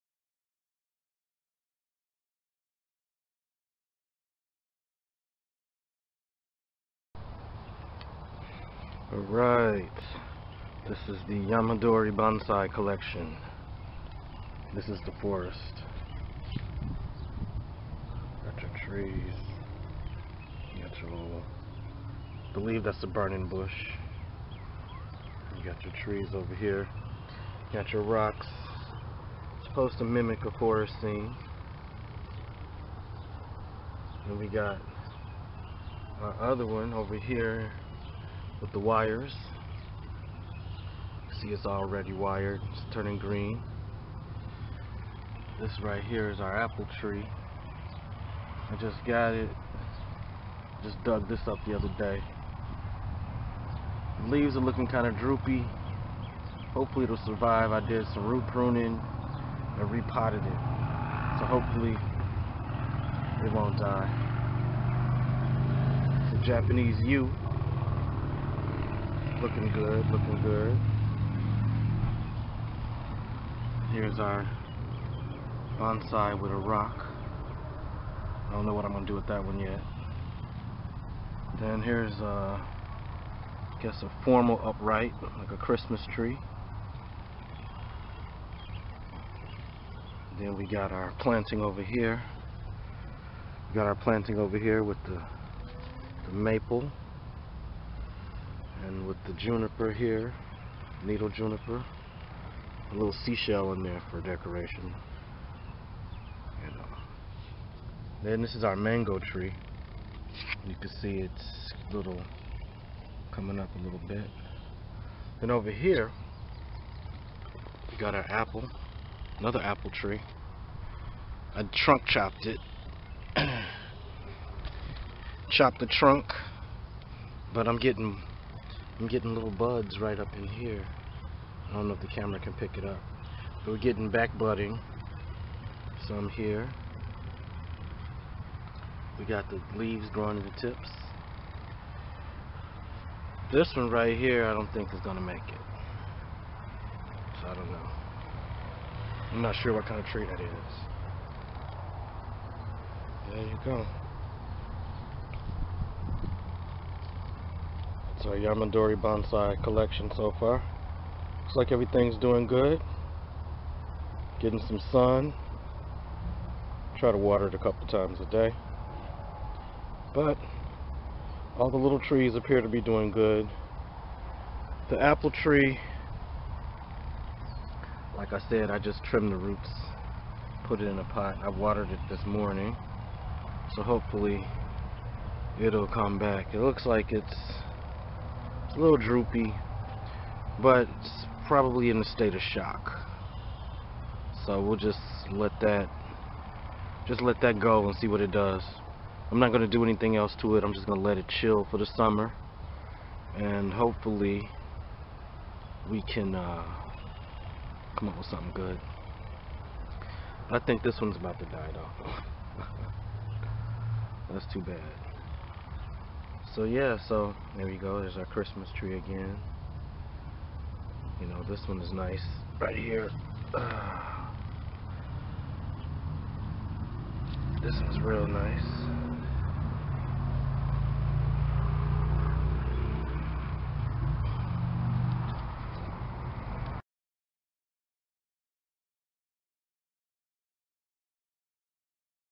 yamadori bonsai 2
Out at my yamadori bonsai bench in the back yard checking on the progress of every one.